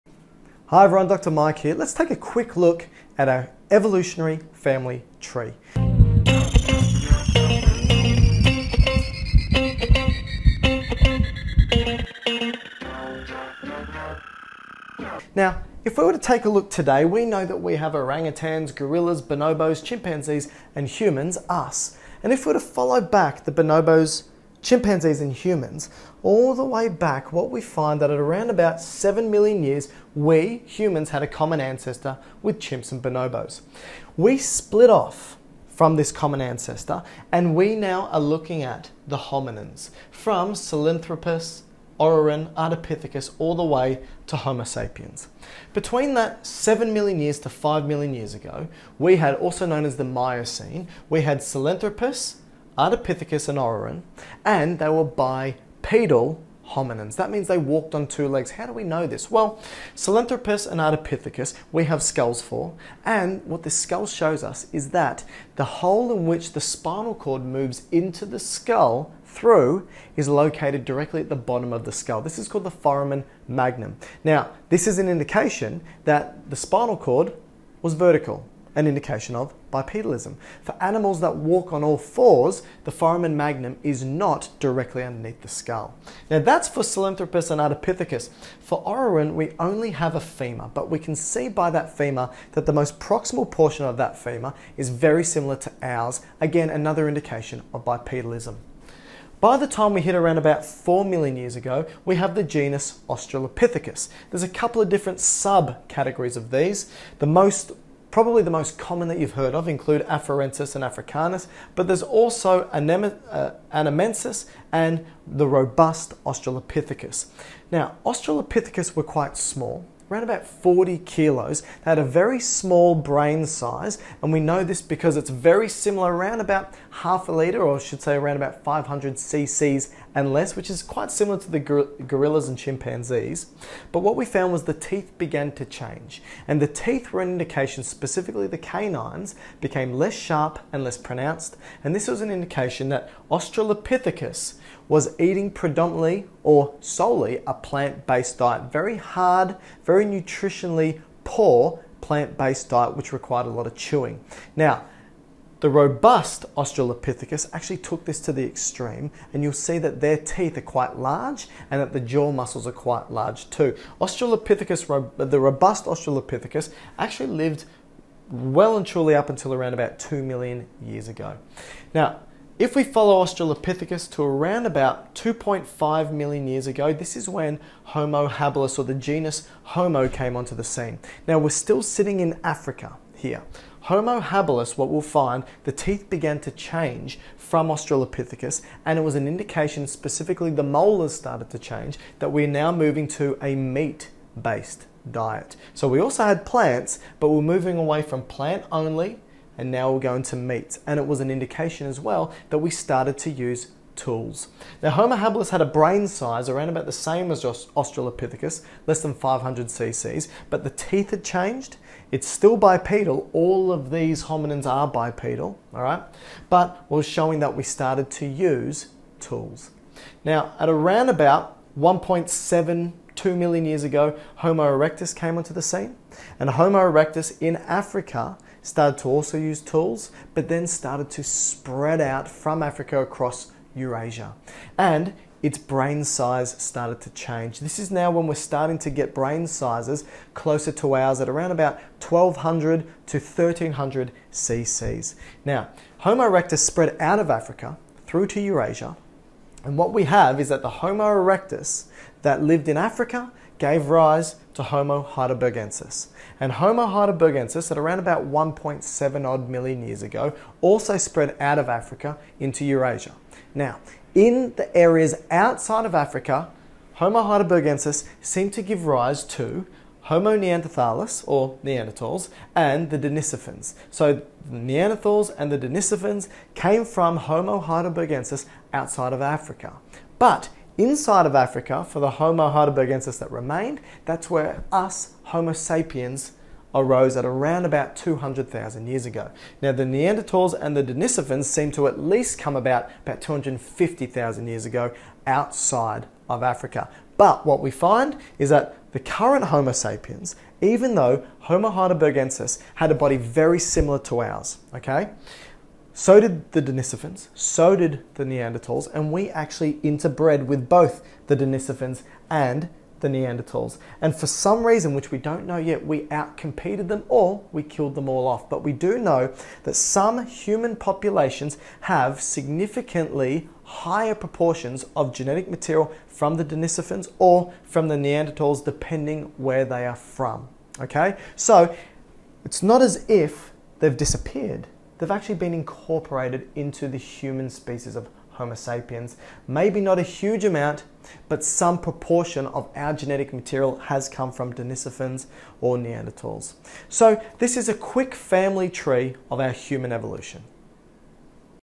Human Evolution
In this video, Dr Mike summarizes 7 million years of human (hominin) evolution in under 10 minutes!!